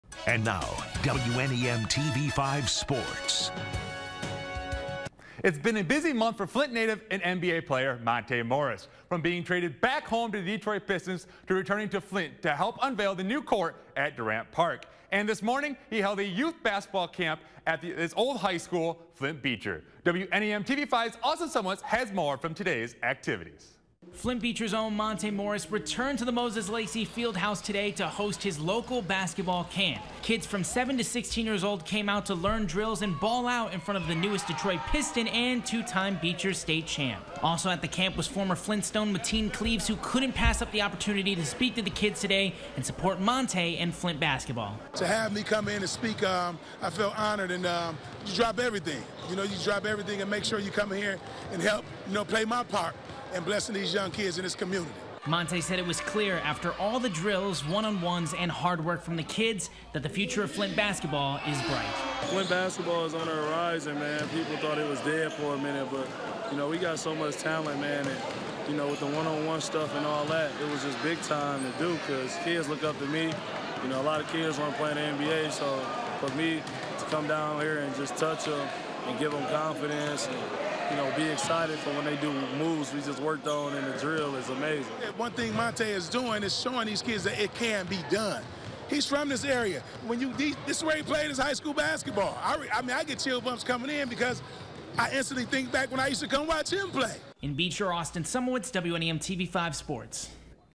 Monte Morris 'Buctown' basketball camp
Two-time state champion and newest Piston hosts camp at Beecher.